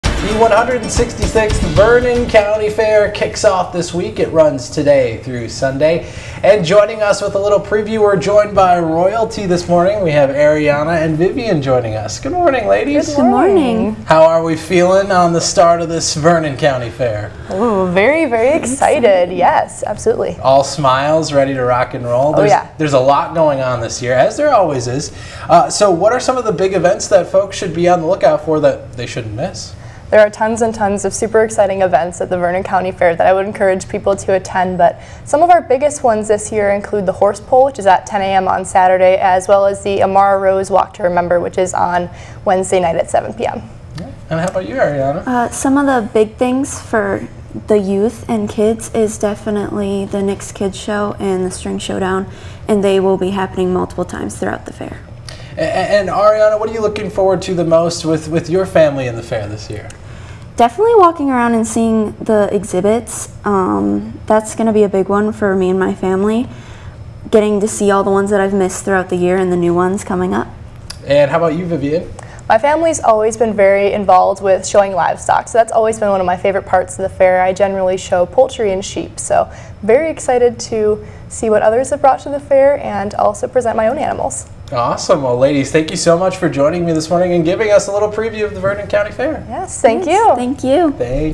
166th Vernon County Fair kicks off Wednesday
The 166th Vernon County Fair takes place September 13-17 at 210 Fairground Rd.